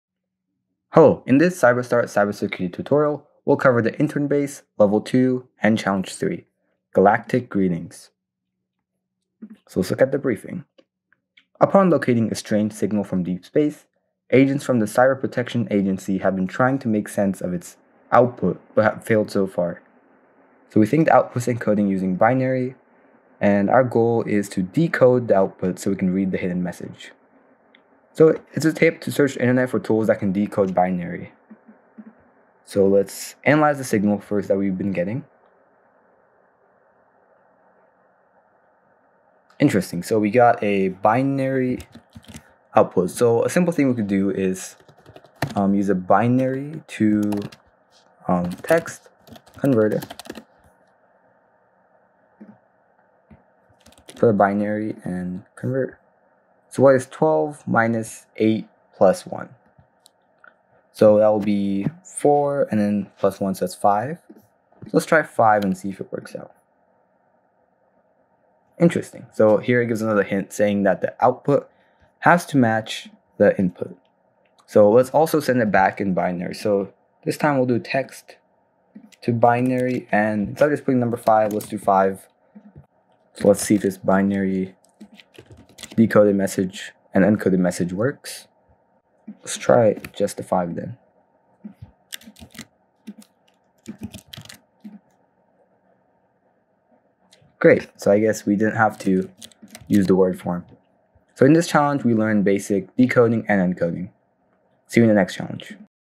CyberStart Intern Base Level 2 Challenge 3 | Galactic Greetings | Cybersecurity
In the walkthrough of the CyberStart America Internbase Level 2 Challenge 3, you will be introduced to encoding and decoding binary. Firstly, I recommend you all try it by yourself, as that is when most learning commonly happens. CyberStart L02:C03 Galactic Greetings
Cybersecurity Challenge (Social Engineering, SQL Injections, Website Dev Tools)
Briefing L02 C03

Galactic Greetings
Upon locating a strange signal from deep space, agents from the Cyber Protection Agency have been trying to make sense of its output but have failed so far. We think that the output is being encoded using Binary. So, we believe that if we can decode the output we will reveal a hidden message! Can you help us do that?

Tip: Search the Internet for tools that can decode binary.

CyberStart Ethical Hacking CTF Walkthrough
Created with permission from CyberStart.com.